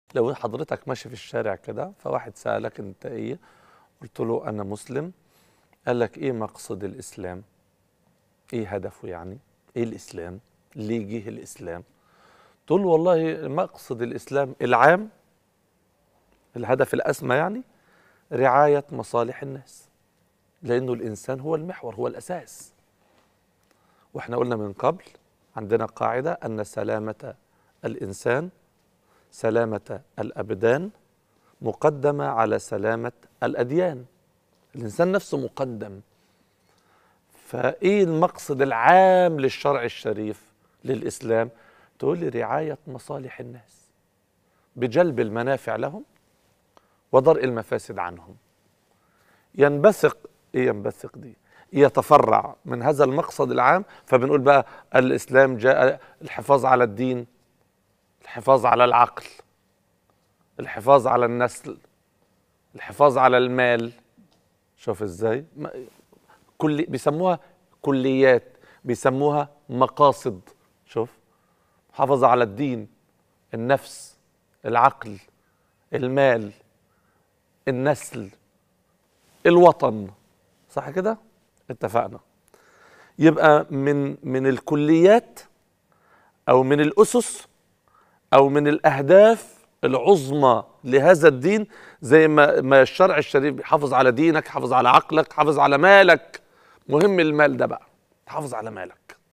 الشيخ رمضان عبد المعز يوضح المقصد العام من الإسلام
"برنامج ديني اجتماعي يقدمه الشيخ خالد الجندي والشيخ رمضان عبد المعز بأسلوب عصري يركز على تجديد الخطاب الديني ويهدف إلى الارتقاء بوعي المسلمين, يذاع من السبت الى الخميس الساعة 5.00 مساءً على dmc